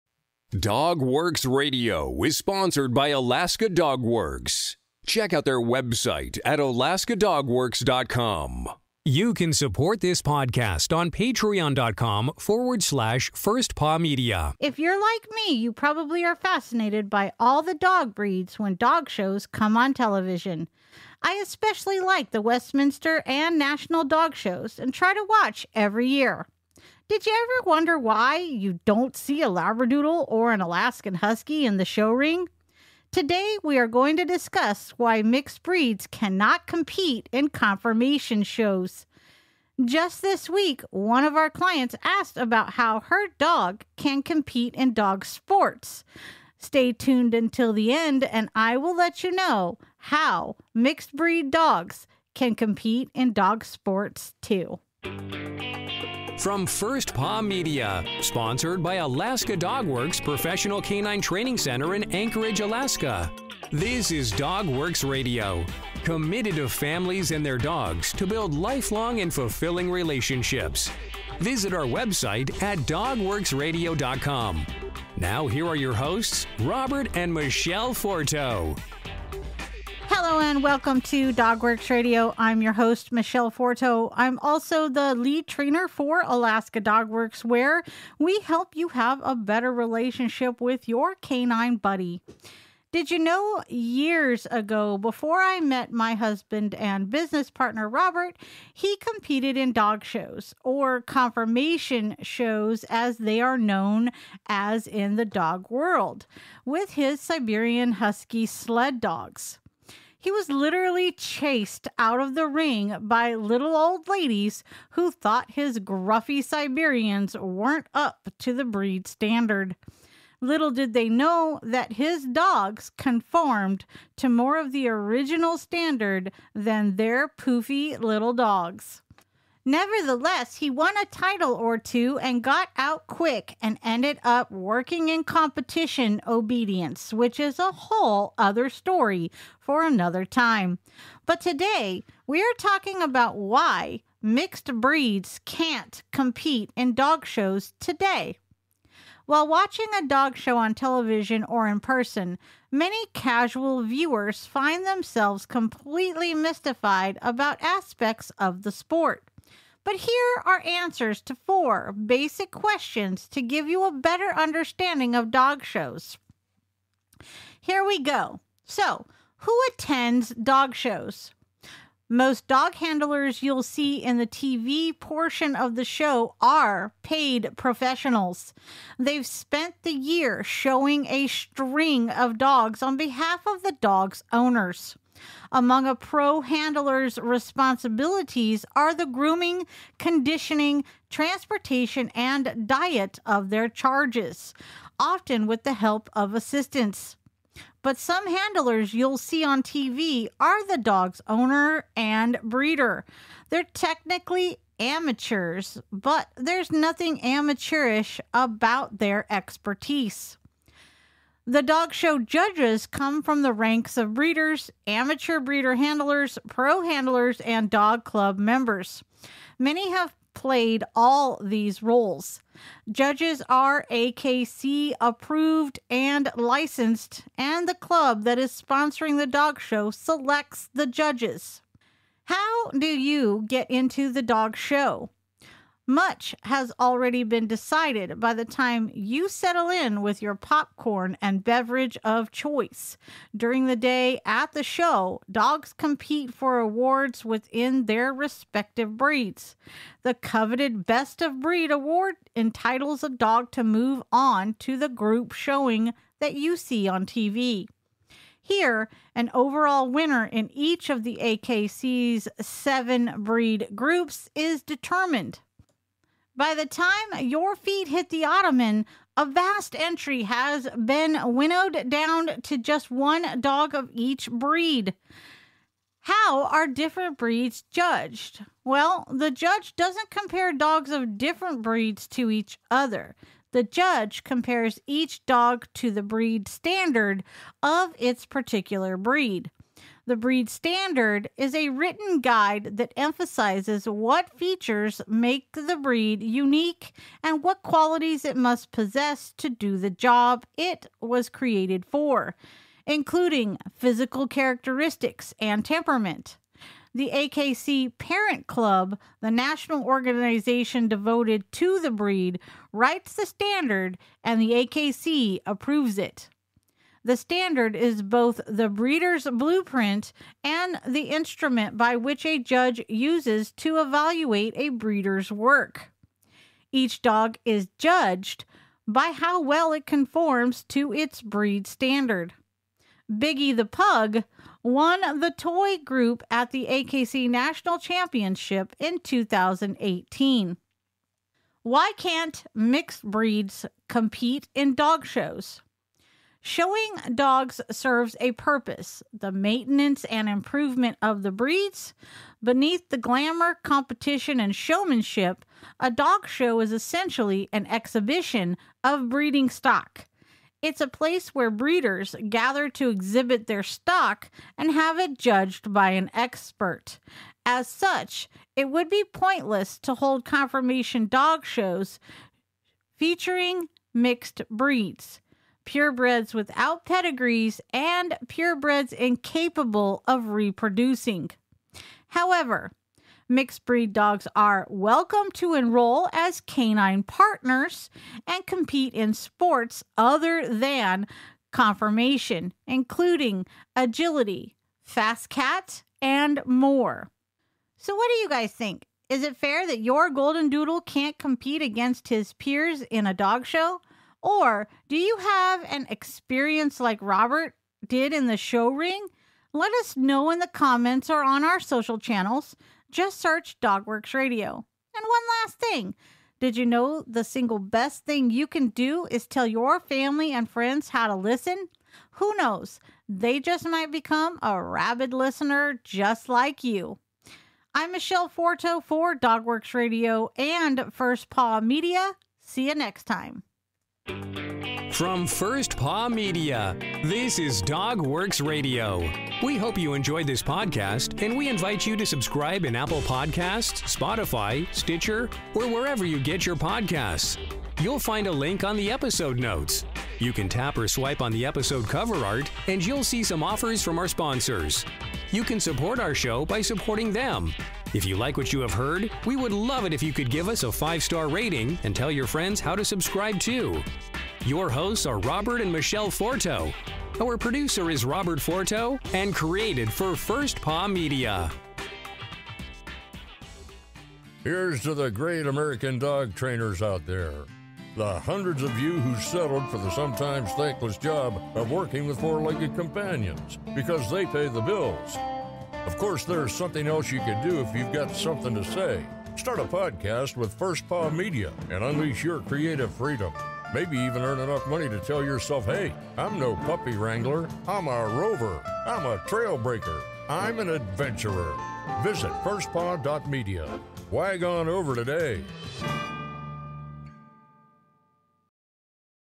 Why can't Mixed Breed Dogs Compete in Dog Shows
Join Michele Forto as she discusses why mixed breed dogs cannot compete in dog shows (conformation).